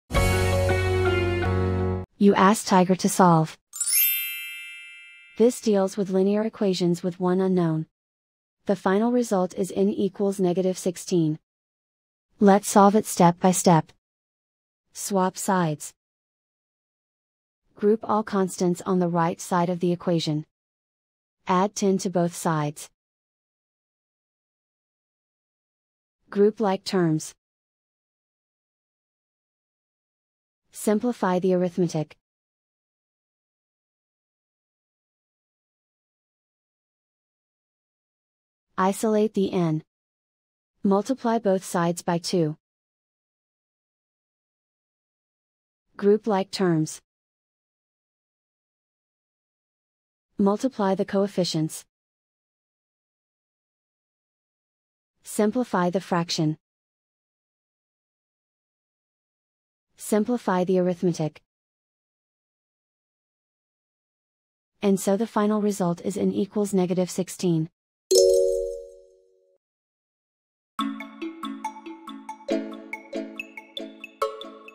Solve -18=-10+n/2: Linear Equation Video Solution | Tiger Algebra
Struggling with -18=-10+n/2? Watch our step-by-step video on Tiger Algebra to master this linear equation effortlessly.